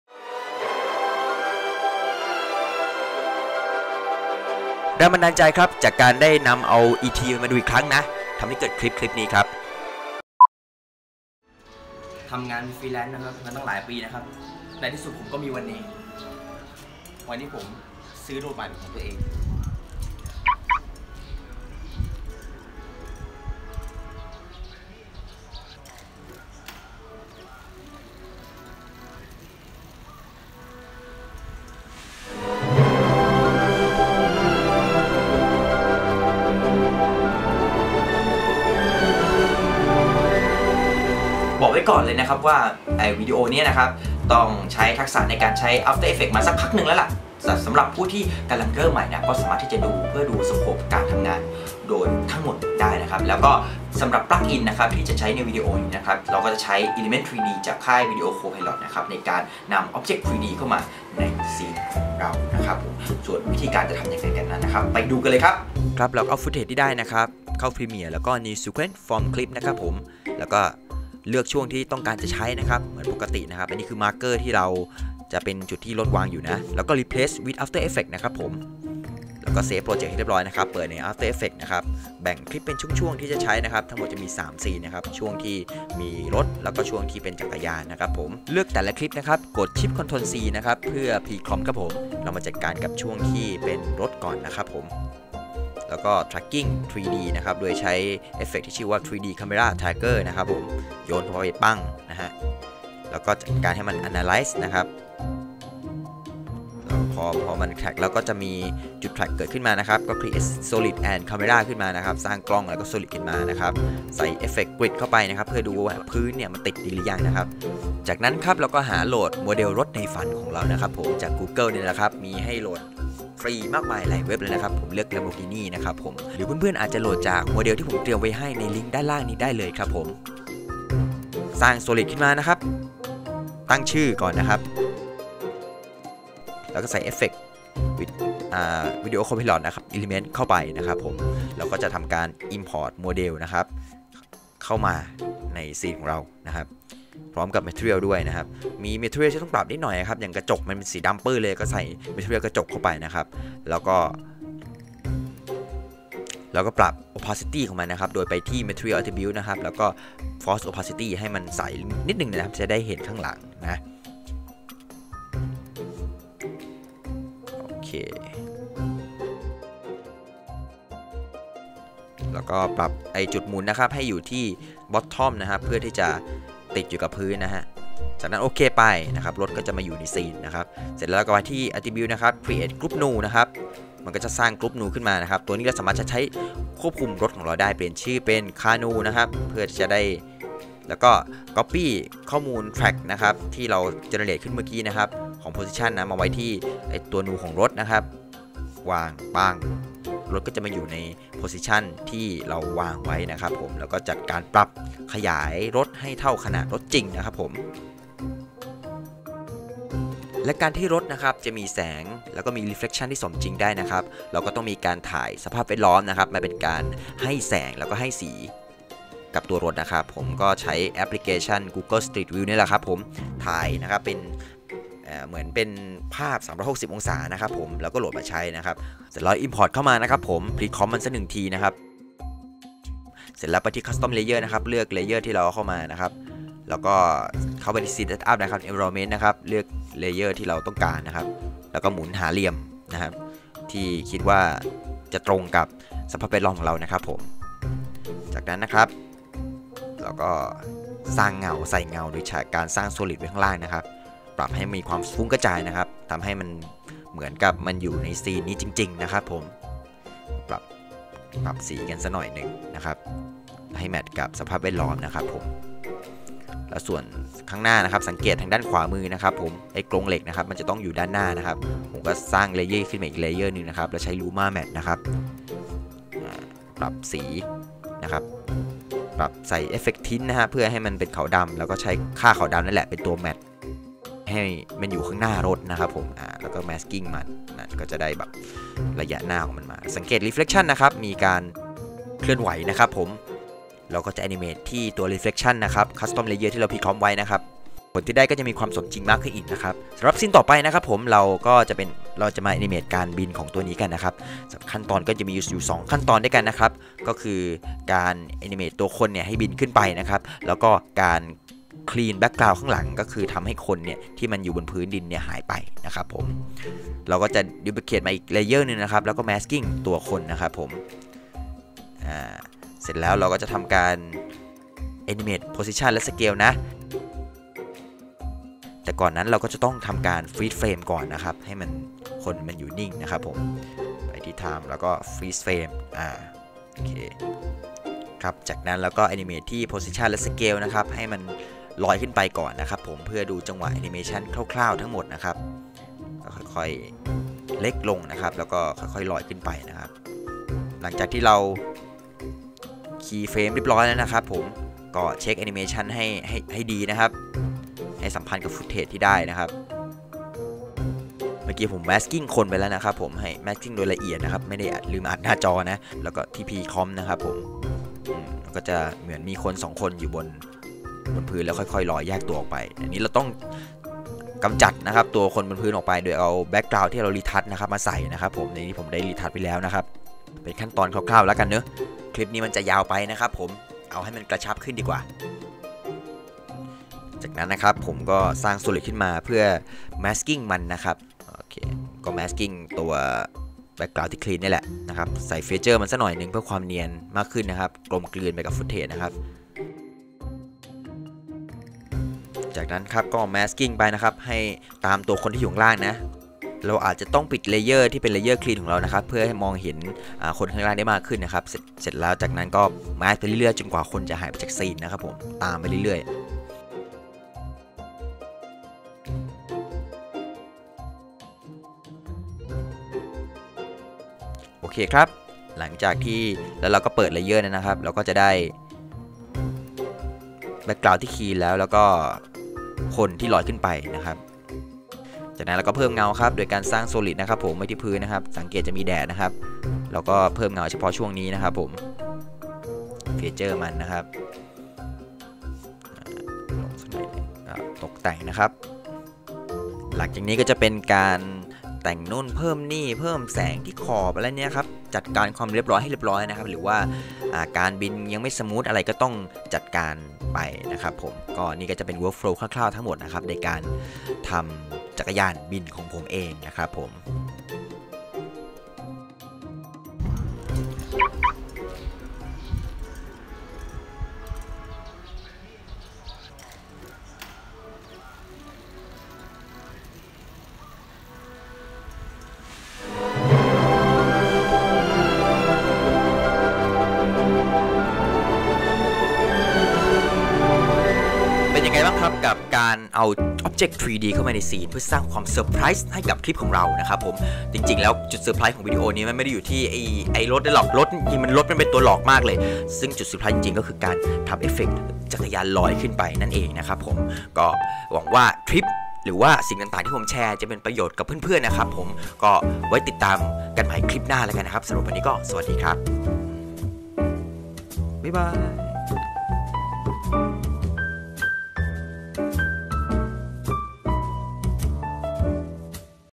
เบื้องหลังรถใหม่ของผม : How to put 3D Lamborghini in After effect scene
This tutorial video contains how to put 3D car in your scene using element 3D in after effect.

The tutorial also includes how to make bicycle flying is a plus
you can download footage and Lamborghini from this link